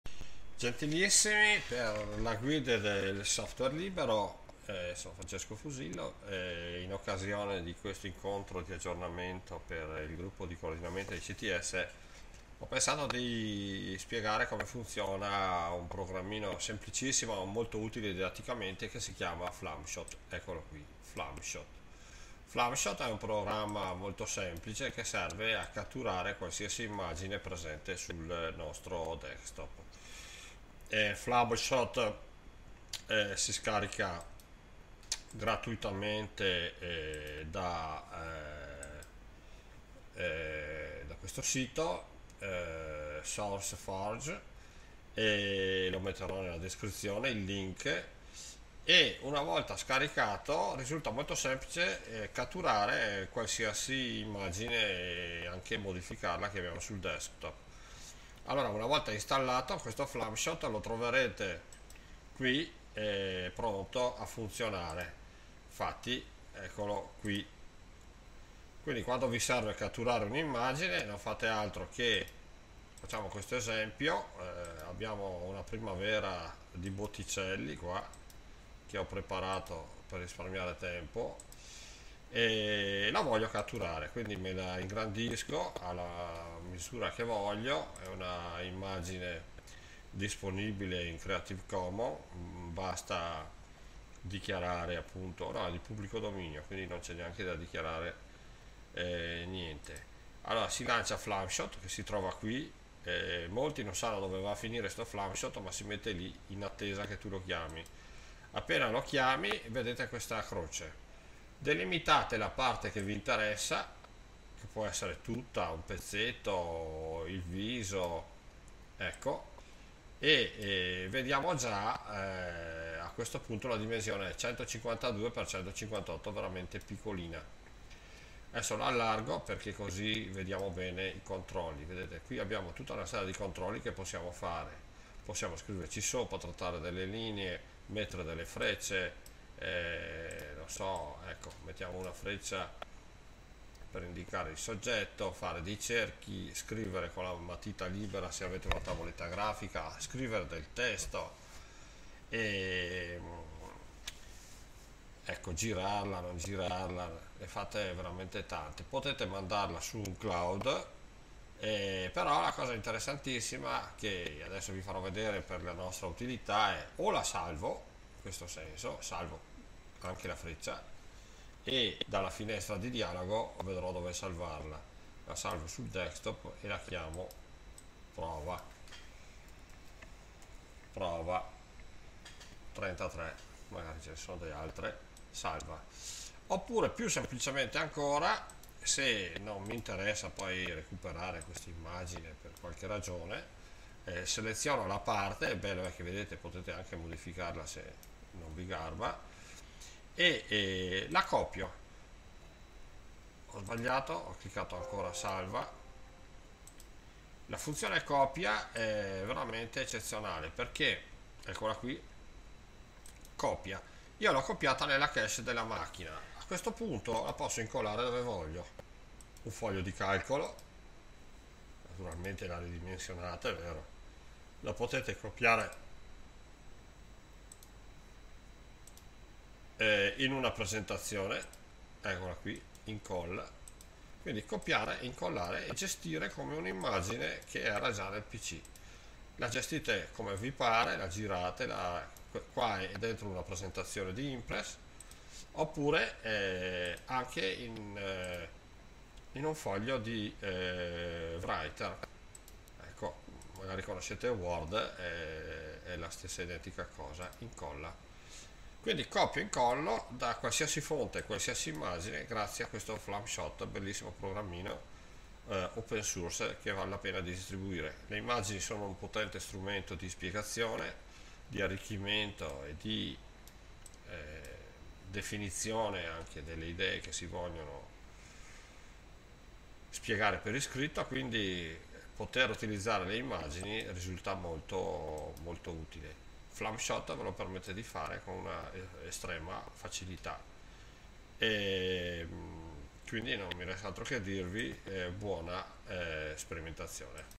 Flameshot videoguida
con flamshot catturi e importi qualsiasi immagine presente sul tuo Deskto.
Open Source e multipiattaforma, anche per LINUX.